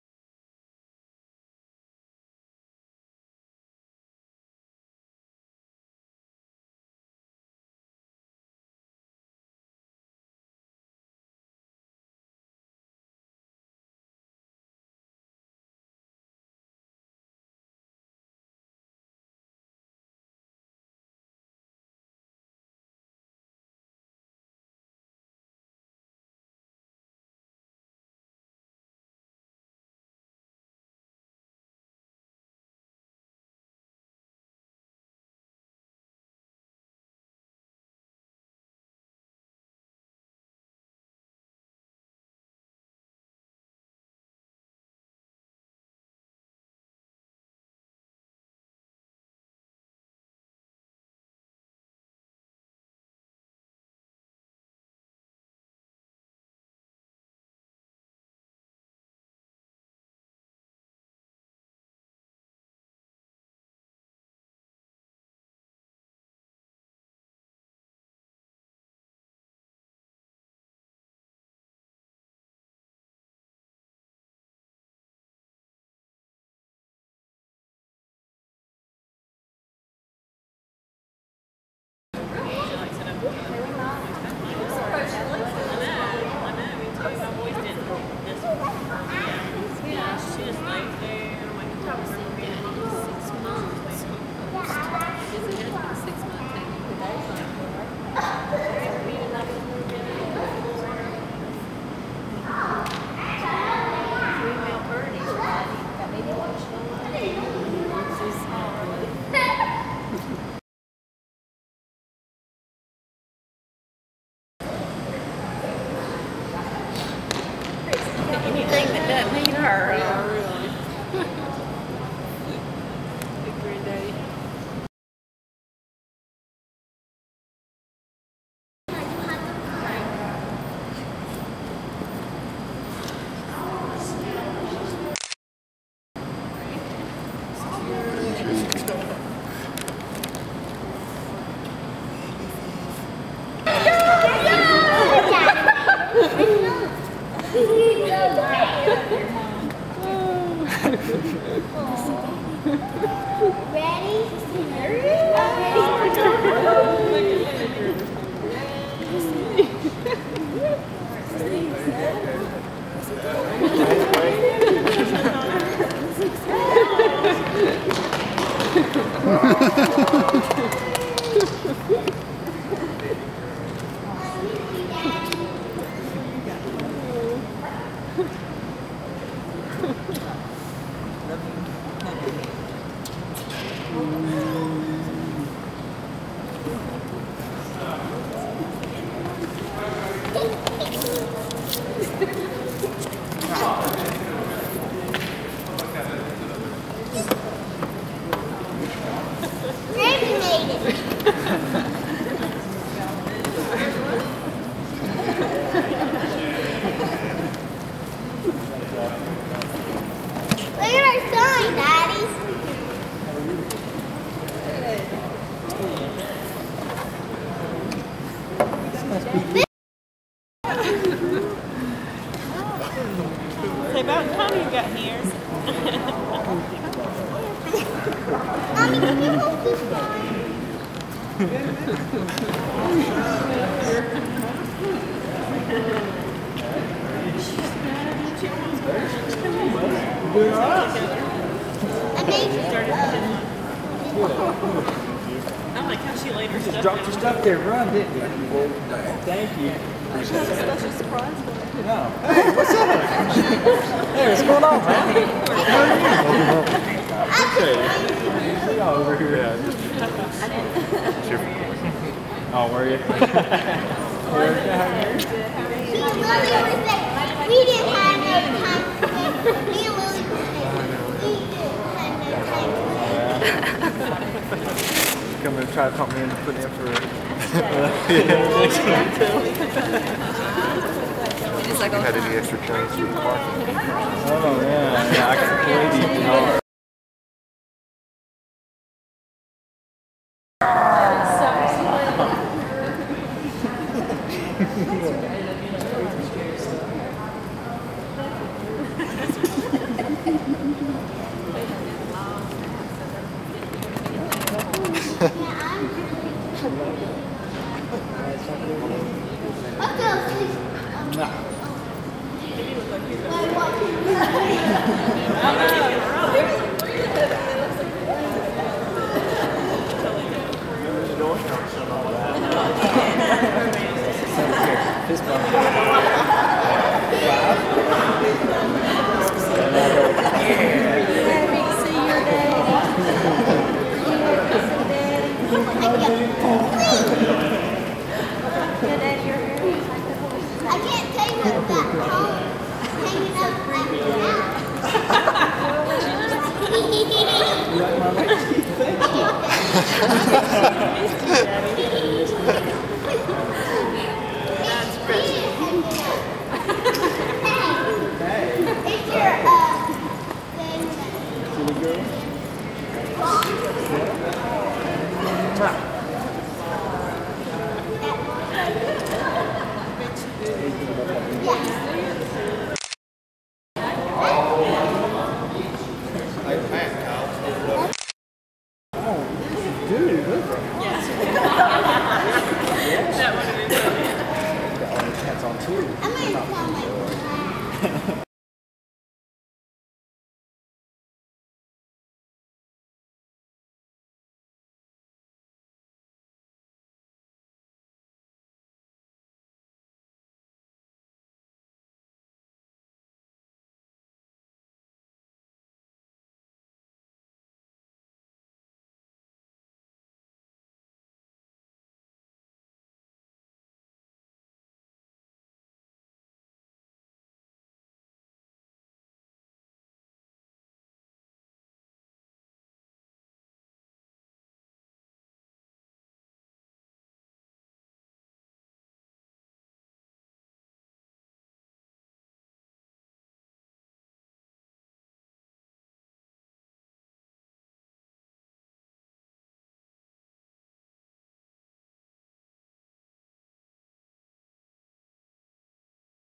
Coit's Homecoming
On November 5, 2011, Coit Dixon and a close friend returned home to their wifes and families after a year in Kuwait and several other Middle Eastern conutries.